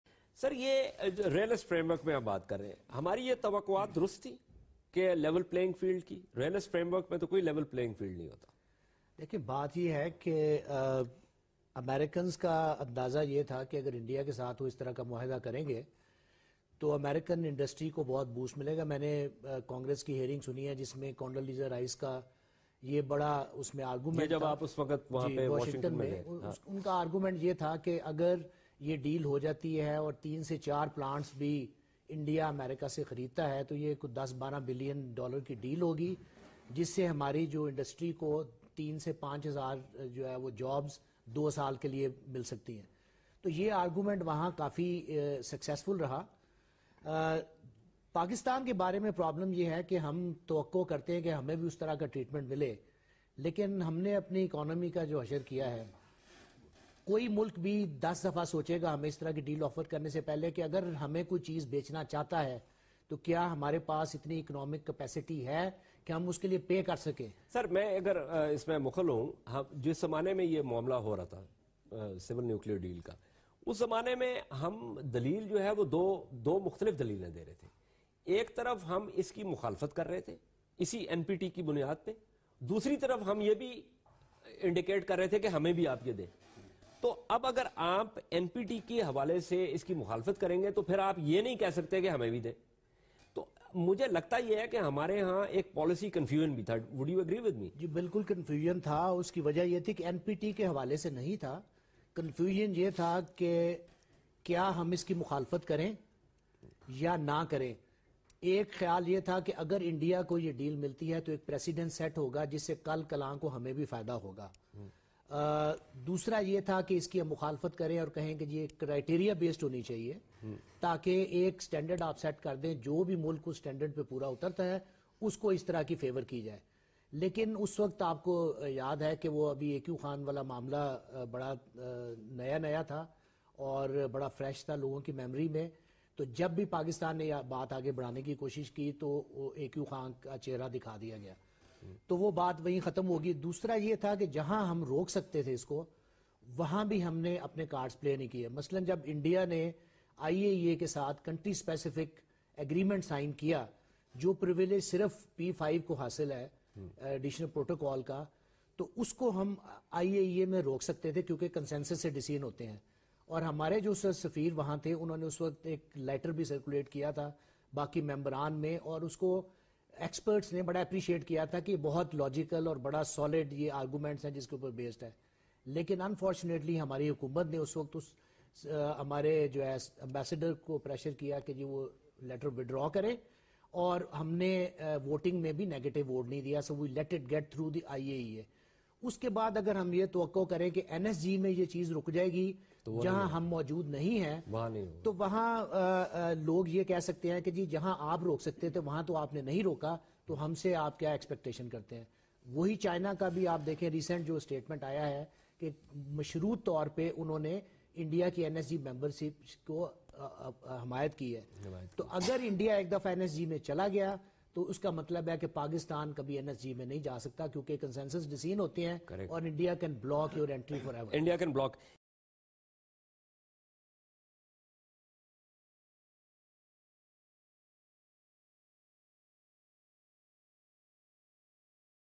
Pakistan Have No Money to do Nuclear Deal Like India
Pakistan has zero chance of getting a US civilian nuclear deal like the Indian one.
In this murky atmosphere, the US is trying to get Pakistan to implement its private agenda. But the  Pew Survey shows fully 64% of Pakistanis view the US as an enemy, and only 9% as a partner. This cannot be the basis of a long term strategic partnership. By contrast, 71% of Indians have a favorable view of the US.
Most Pakistanis say the US has simply used them and then dumped them when convenient. Very true. But that underlines the absence of a long-term strategic fit. Which is why Pakistan cannot hope to get nuclear parity with India.